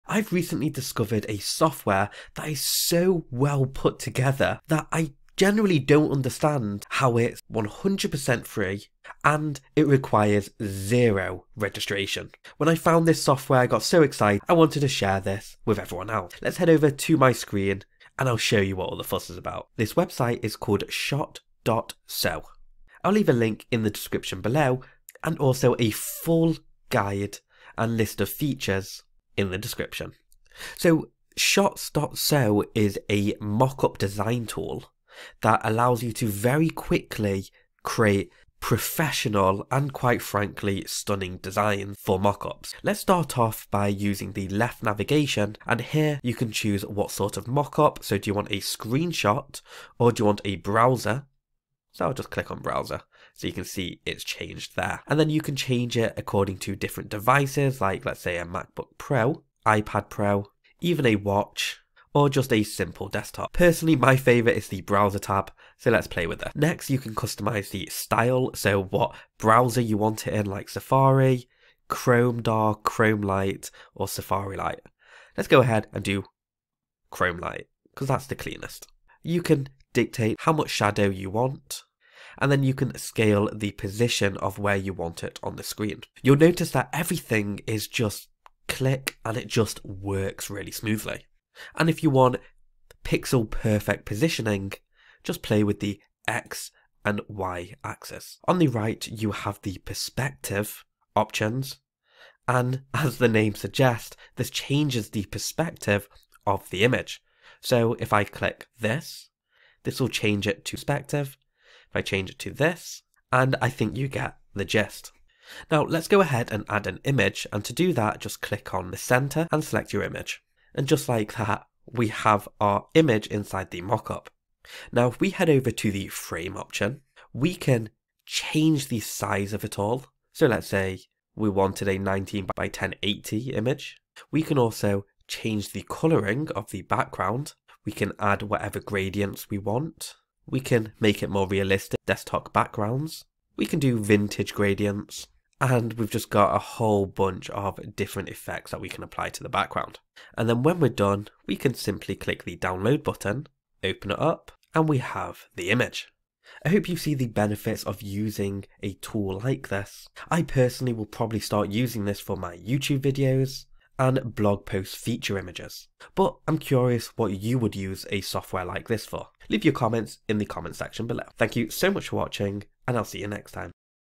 Free 1-Click Software - Shots So Demo
Looking for a free mockup design tool that's easy to use and doesn't require any registration? Look no further! This amazing new tool is perfect for anyone who wants to create stunning designs without the hassle of complicated software or sign-up processes.

With shots so mockup design tool, you'll be able to create beautiful mockups in minutes, whether you're a seasoned designer or a complete novice. And the best part? You don't need to pay a penny or register for anything to get started!

So what are you waiting for? Start designing today with this incredible mockup design tool and watch your creativity soar! Whether you're creating designs for a personal project, a business, or just for fun, you'll love the ease and simplicity of this free tool. So why not give it a try and see for yourself?

🏆 My Goal 🏆

👨 About Me 👨